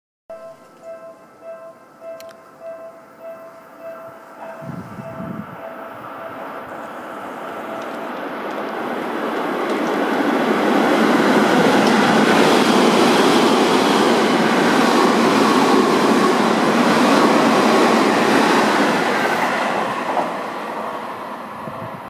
【フルHD】西武鉄道池袋線6000系(準急) 富士見台(SI08)駅通過
6158F(相互直通運転開始HM&ラピ)。
相互直通運転開始の記念HMを掲出した6000系の池袋行きの準急です。これはこれでちょっとレアかも・・ｗ